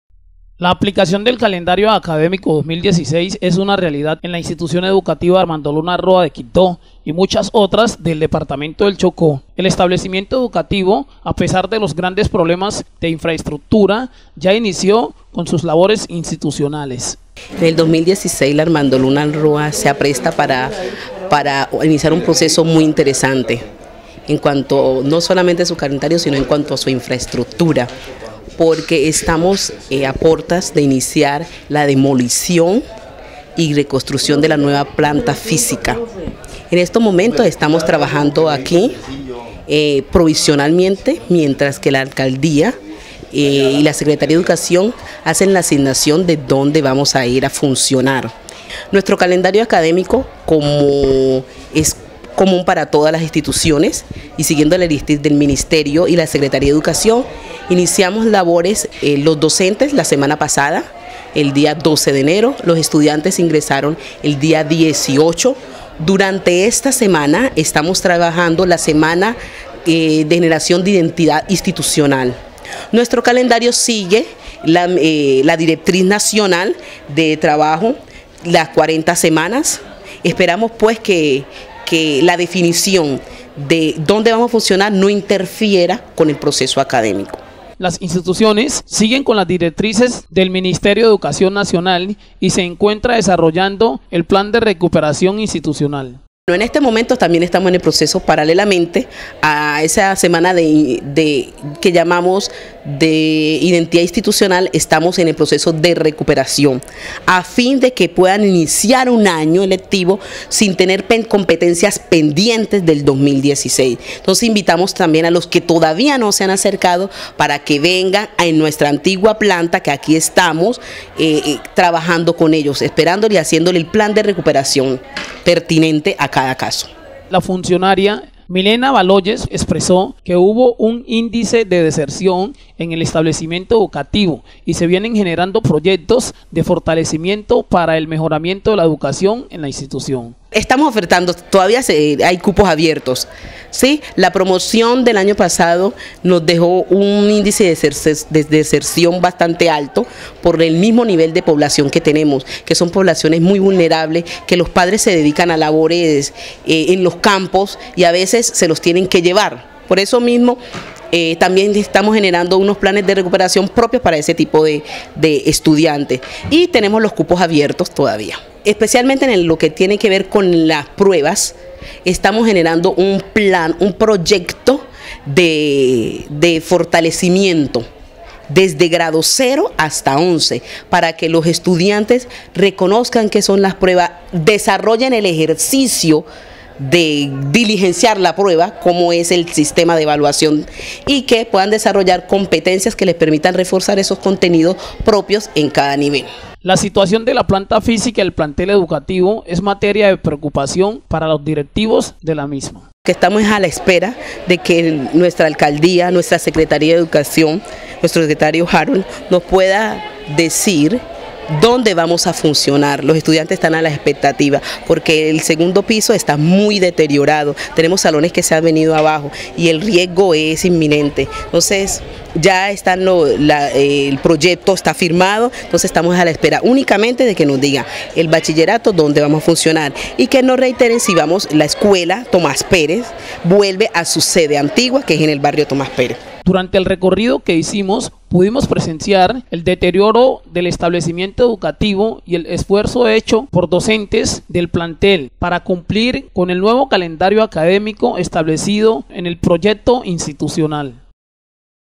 INSTITUCIONES EDUCATIVAS DAN CUMPLIMIENTO AL CALENDARIO ACADÉMICO 2016
La aplicación del calendario académico 2016 es una realidad en la institución educativa armando luna roa de Quibdó, y muchas otras del departamento del choco, el establecimiento educativo a pesar de los grandes problemas  de infraestructura ya  inicio  con sus labores institucionales
la funcionaria expreso que hubo un índice de deserción en el establecimiento educativo y se vienen generando proyectos de fortalecimiento para el mejoramiento de la educación en la institución
La situación de la planta física del plantel educativo  es materia de preocupación para los directivos de la institución ,se espera el apoyo de la nueva administración municipal
Durante el recorrido que hicimos pudimos presenciar el deterioro del establecimiento educativo y el  esfuerzo hecho por los docentes del plantel educativo para cumplir con el nuevo calendario académico, establecido en el proyecto institucional .